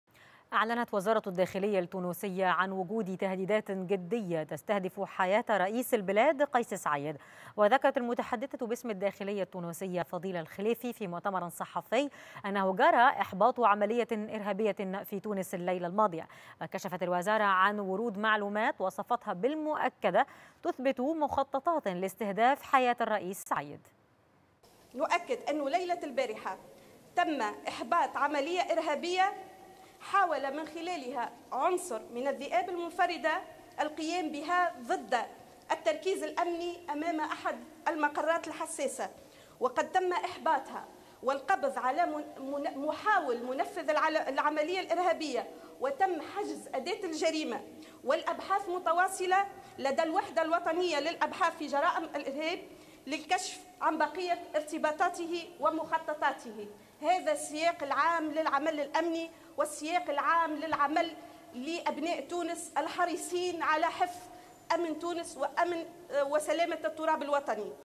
غرفة الأخبار| الداخلية التونسية تعلن إحباط محاولة إرهابية استهدفت مؤسسة الرئاسة
24  يونيو 2022